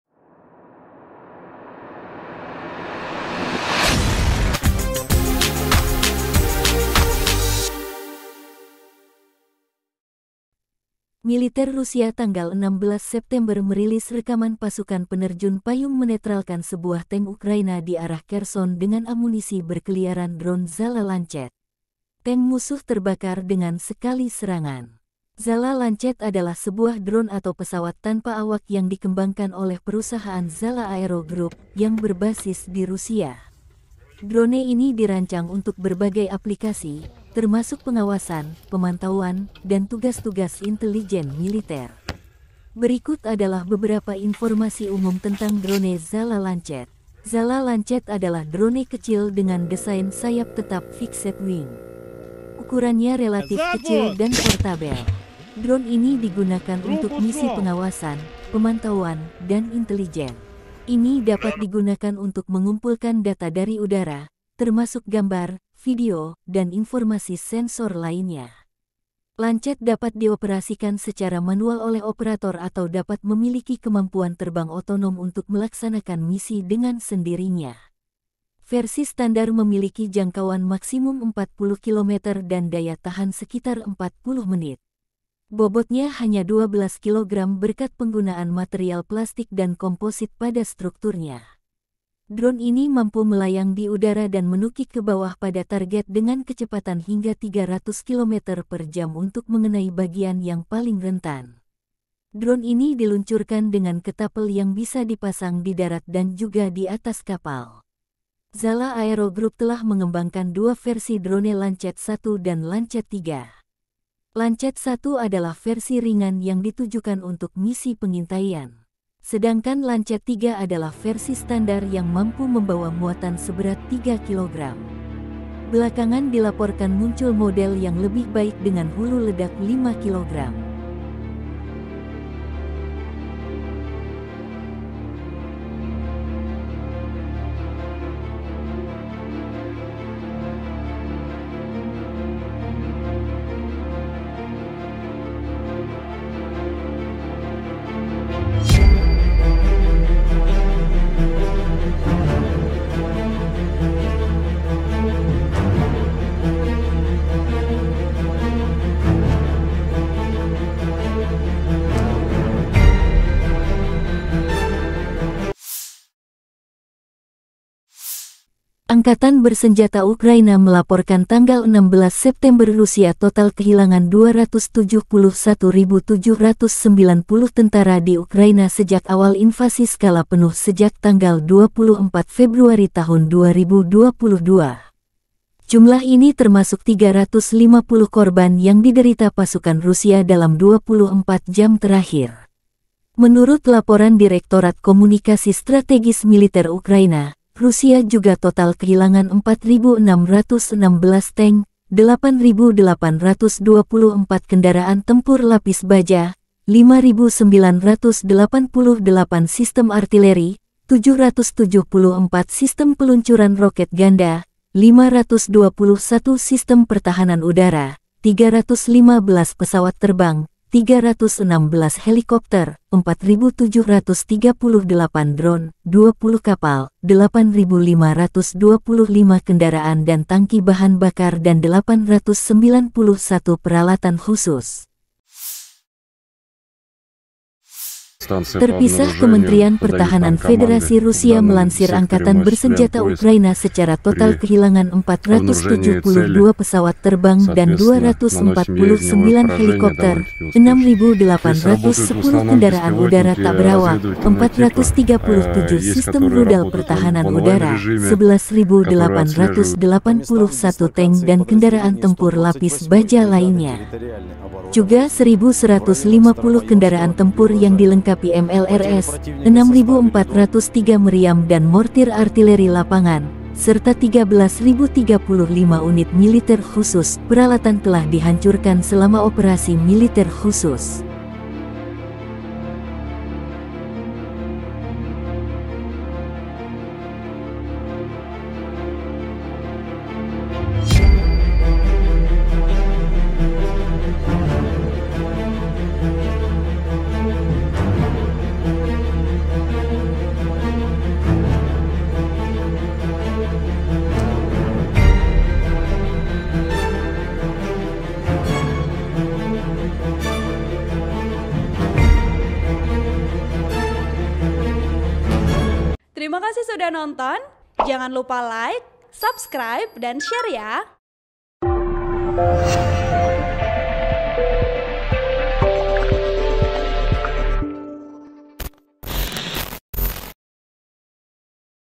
🔴 FULL Video Drone Lancet Rusia Hantam Tank Ukraina di Garis Depan, Pasukan Zelenskiy Rugi
TRIBUN-TIMUR.COM - Militer Rusia 16 September merilis rekaman pasukan penerjun payung menetralkan sebuah tank ukraina di arah kherson dengan amunisi berkeliaran dron zala lancet.

Tank musuh terbakar dengan sekali serangan.
Zala Lancet adalah sebuah dron atau pesawat tanpa awak yang dikembangkan oleh perusahaan Zala Aero Group, yang berbasis di Rusia.

Drone ini dirancang untuk berbagai aplikasi, termasuk pengawasan, pemantauan, dan tugas-tugas intelijen militer. Berikut adalah beberapa informasi umum tentang drone Zala Lancet:

Zala Lancet adalah drone kecil dengan desain sayap tetap (fixed-wing). Ukurannya relatif kecil dan portabel.

Dron ini digunakan untuk misi pengawasan, pemantauan, dan intelijen. Ini dapat digunakan untuk mengumpulkan data dari udara, termasuk gambar, video, dan informasi sensor lainnya.
Lancet dapat dioperasikan secara manual oleh operator atau dapat memiliki kemampuan terbang otonom untuk melaksanakan misi dengan sendirinya.

Versi standar memiliki jangkauan maksimum 40 km dan daya tahan sekitar 40 menit. Bobotnya hanya 12 kg berkat penggunaan material plastik dan komposit pada strukturnya. Dron ini mampu melayang di udara dan menukik ke bawah pada target dengan kecepatan hingga 300 km/jam untuk mengenai bagian yang paling rentan. Dron ini diluncurkan dengan ketapel yang bisa dipasang di darat dan juga di atas kapal.

Zala Aero Group telah mengembangkan dua versi drone Lancet-1 dan Lancet-3. Lancet-1 adalah versi ringan yang ditujukan untuk misi pengintaian. Sedangkan Lancet-3 adalah versi standar yang mampu membawa muatan seberat 3 kg. Belakangan dilaporkan muncul model yang lebih baik dengan hulu ledak 5 kg.(*)